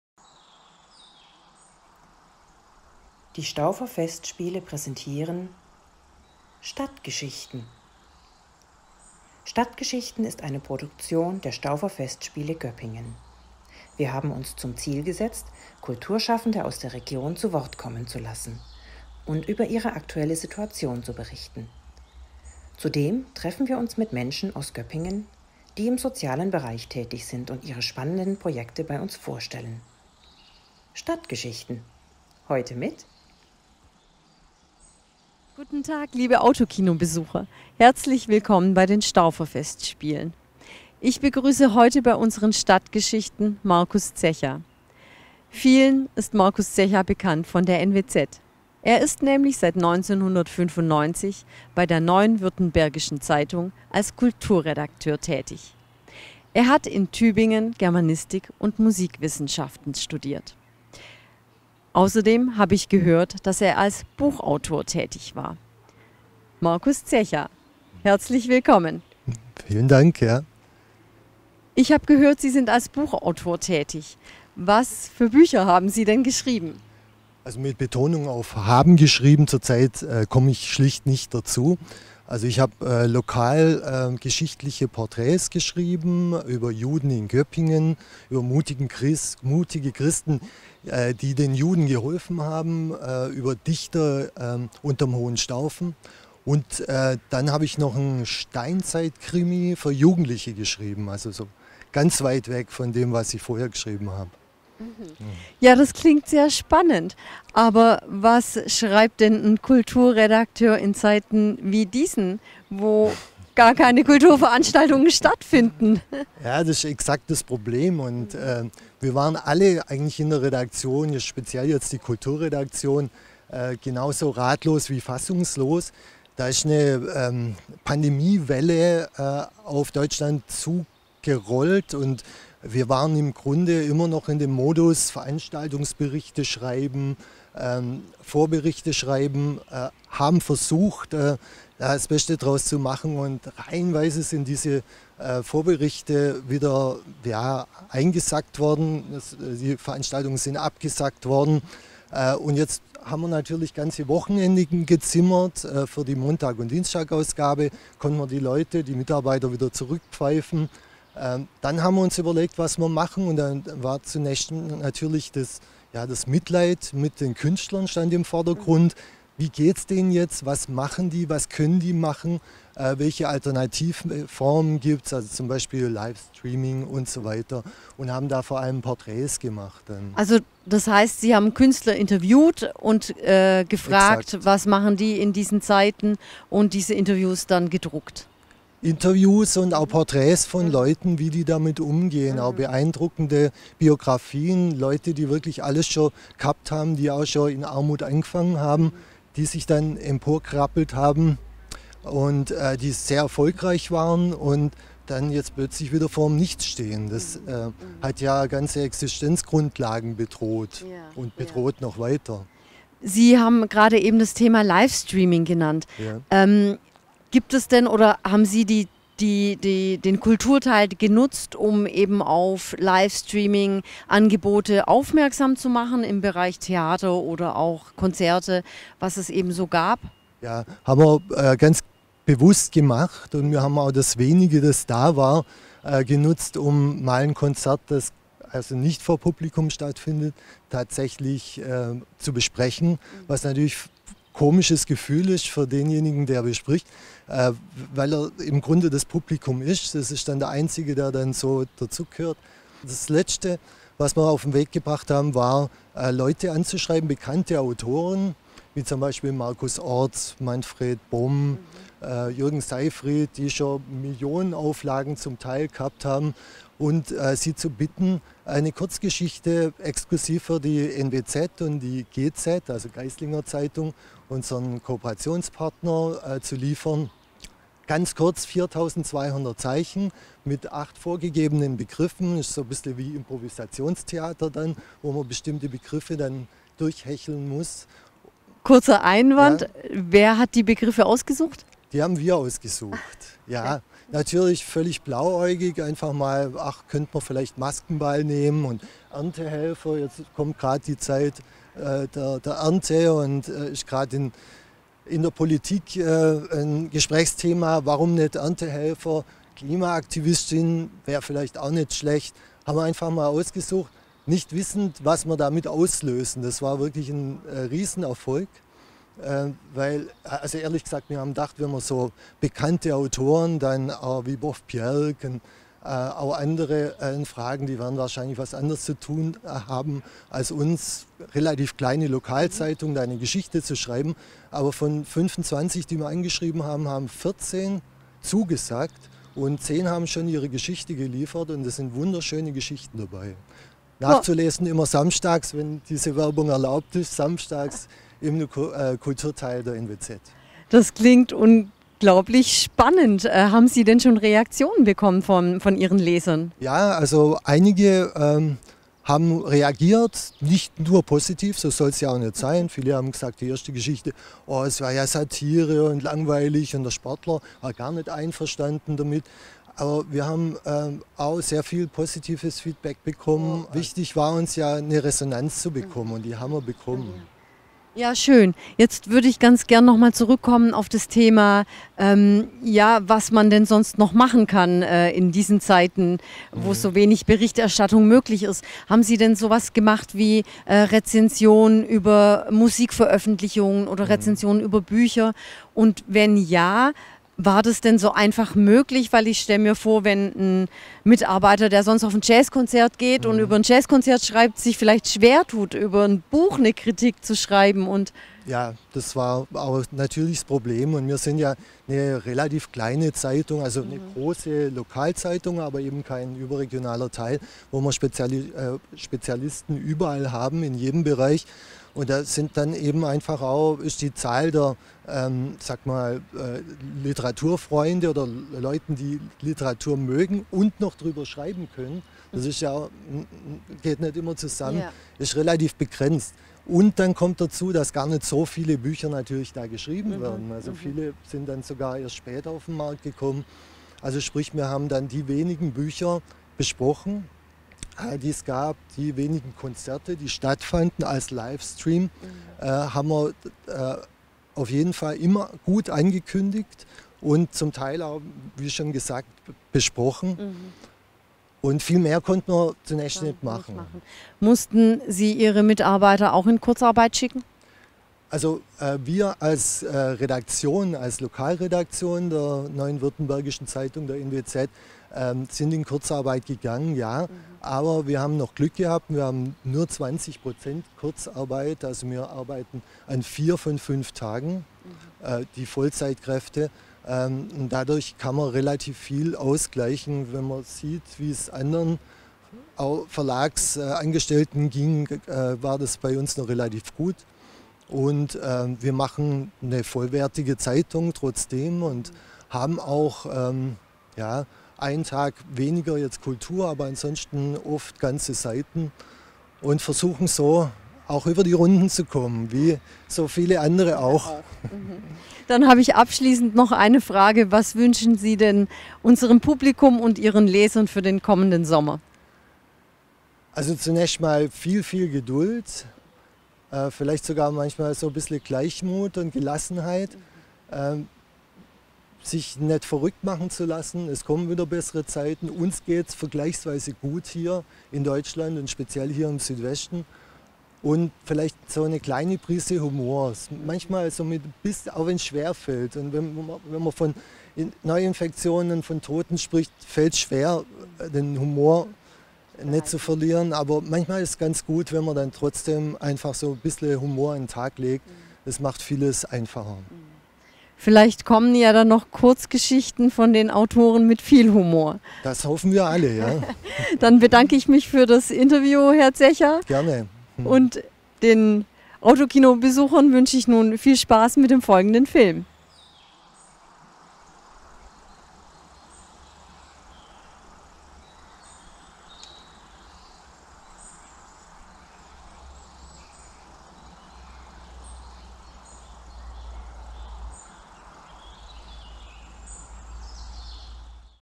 StadtGeschichten m. Marcus Zecha, Kulturredakteur der Neuen Württembergischen Zeitung NWZ, Göppingen
Marcus Zecha ist seit vielen Jahren Kulturredakteur bei der NWZ in Göppingen und erzählt, wie der Lockdown während der Corona-Pandemie seine Arbeit als Journalist beschränkt und wie er und seine Kollegen trotzdem versuchen Kultur für die Leser der NWZ zu schaffen. Marcus Zecha ist außerdem als Buchautor tätig.